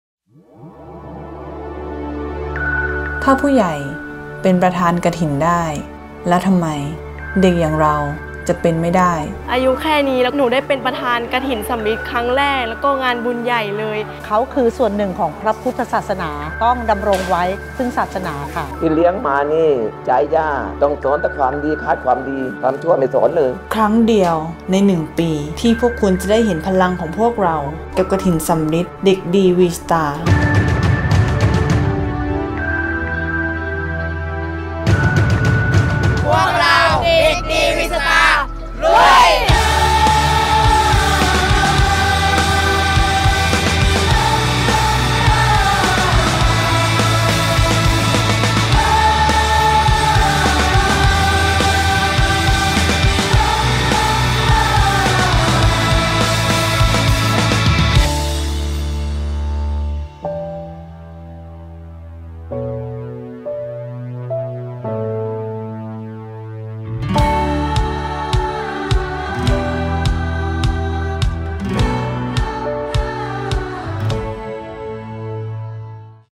TEASER กฐินสัมฤทธิ์เด็กดี V-Star 2562 โรงเรียนศรีเทพประชาสรรค์ จ.เพชรบูรณ์
TEASER กฐินสัมฤทธิ์ เด็กดี V-Star ประจำปี 2562
โรงเรียนศรีเทพประชาสรรค์ จ.เพชรบูรณ์
ติดตามชม Scoop ฉบับเต็มได้ เร็วๆนี้
ผลิตรายการโดย KIDYAI GYDI TEAM | CMY | ชมรมพุทธศาสตร์สากลฯ